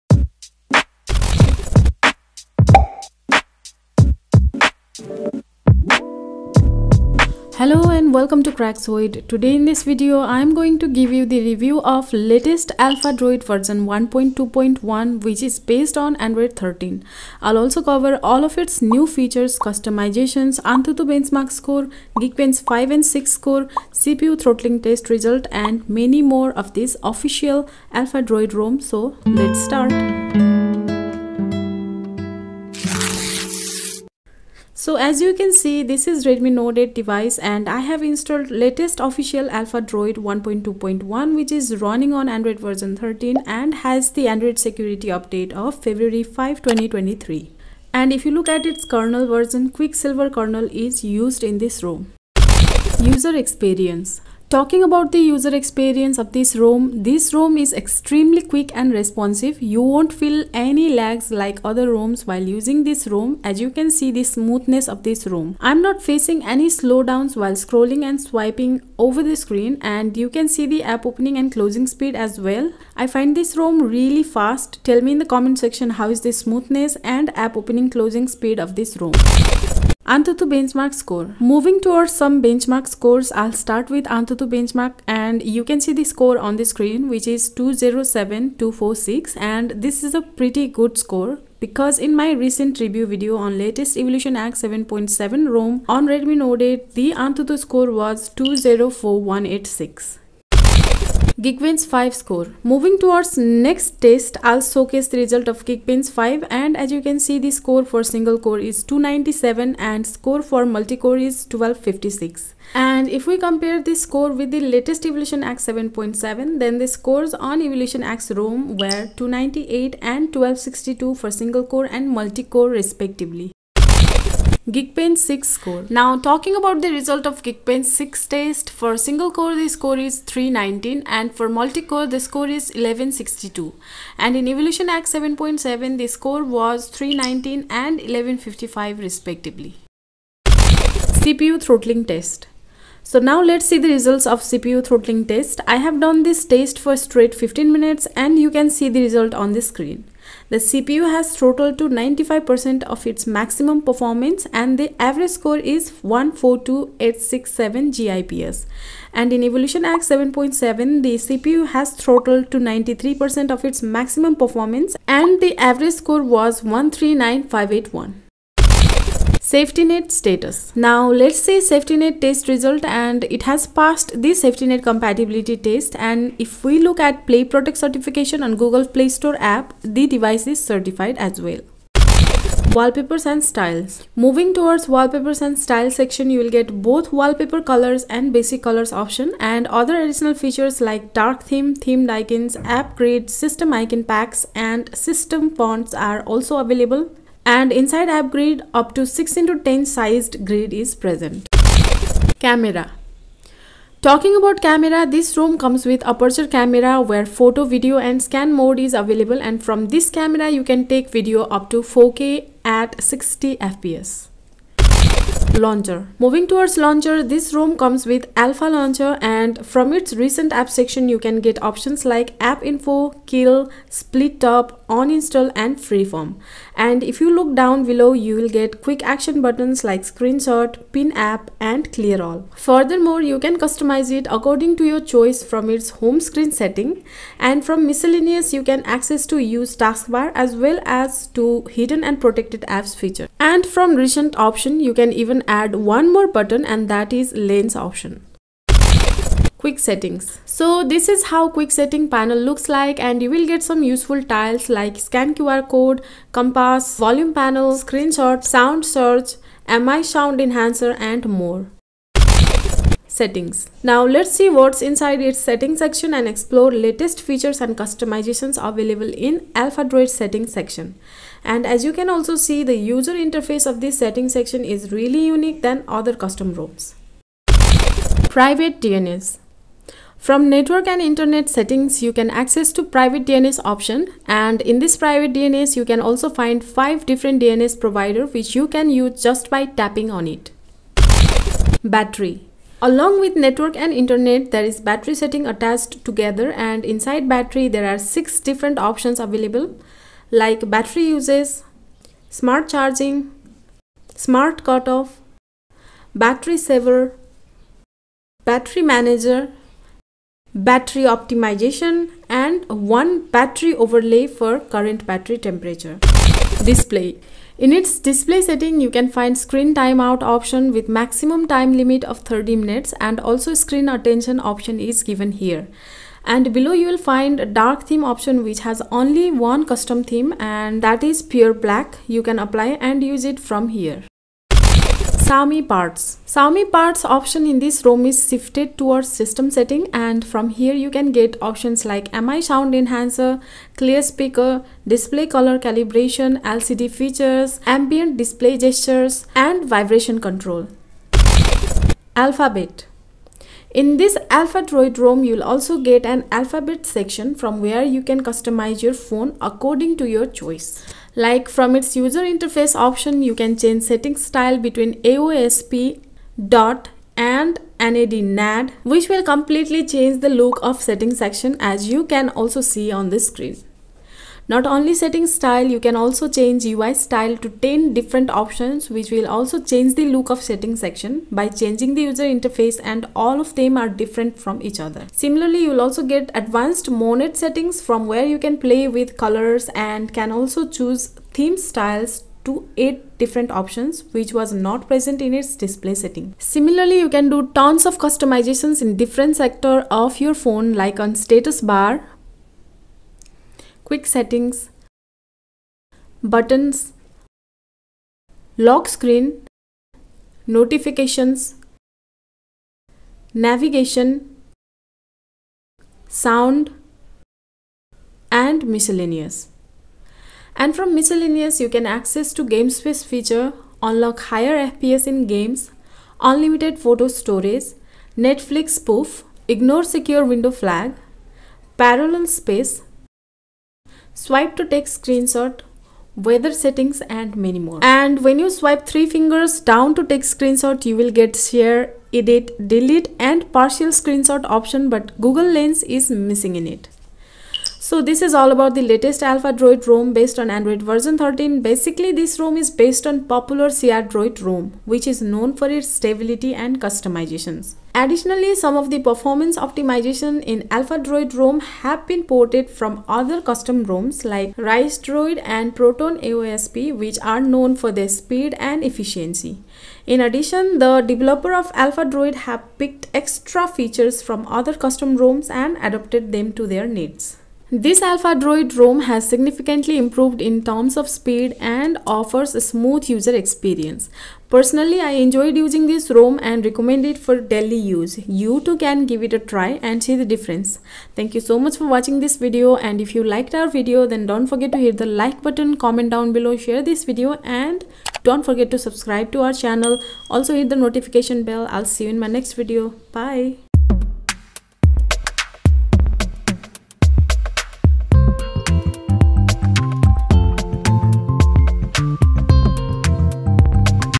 Next Level ROM🔥ft. AphaDroid V1.2.1 ROM Review || Redmi Note 8/8T 🔥🔥
Hello and welcome to Craxoid. Today in this video I will give the review of latest Alphadroid ROM V1.2.1. This ROM comes with lots of next level features and customizations. This Alpha Droid ROM is really smooth and has better performance than other custom roms and I am exploring everything about this ROM like I will show its new features, customizations ,AnTuTu Benchmark Score, Geekbench 5 Score, Geekbench 6 score, CPU Throttling Test,Safetynet status and many more including my personal thoughts related to this Alphadroid ROM.

Timestamp:

0:00 Intro
0:32 About Alpha Droid ROM
0:52 User Experience
1:19 AnTuTu Benchmark Score
1:40 Geekbench 5 Score
2:03 Geekbench 6 Score
2:20 CPU Throttling Test
2:48 Safety Net/CTS
3:01 Wallpaper and Style
3:21 Camera
3:34 Launcher
4:08 Quick Settings
4:22 Settings
4:37 Private DNS
4:50 Battery
5:15 Display
5:34 Xiaomi Parts
5:51 Alphabet (Alphadroid Customizations)
7:27 Conclusion